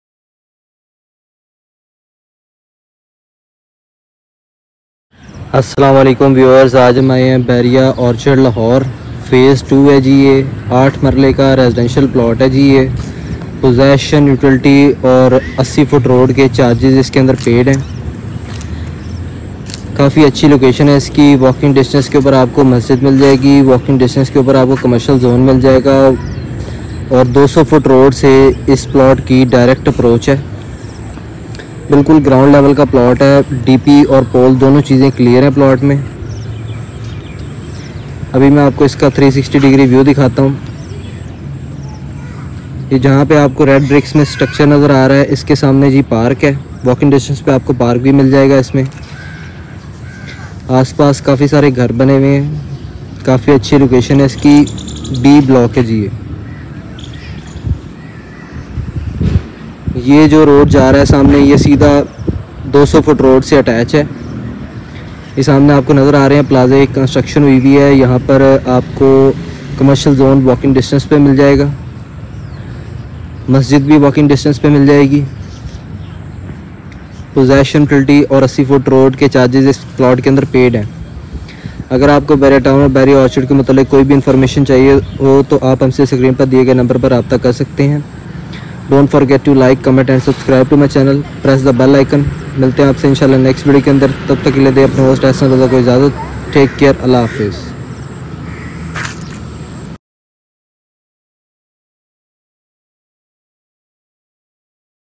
8 Marla Main Bulavard Plot For Sale In B Block Bahria Orchard Phase 2 Lahore
8 Marla Main Bulavard Plot For Sale In B Block Bahria Orchard Phase 2 Lahore
Possession Paid
Utility Paid
Main Bulavard Paid
Ahsan Raza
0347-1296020